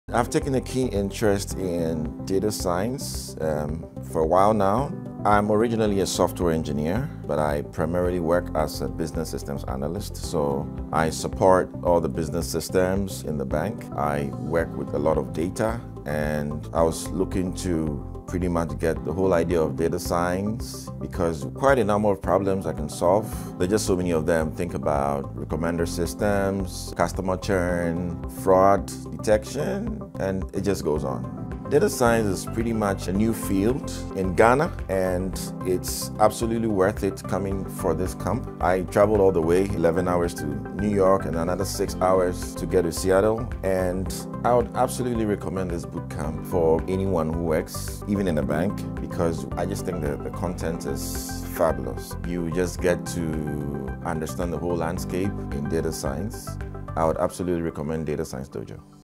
FROM SOFTWARE ENGINEER TO SYSTEM ANALYST I Ep. 6 Emmanuel Tachie-Menson (re-uploaded)
"In our How I Changed My Career Series, we introducing you to people who took a chance and changed their jobs. Meet Emmanuel Tachie-Menson, Coding Dojo's graduate.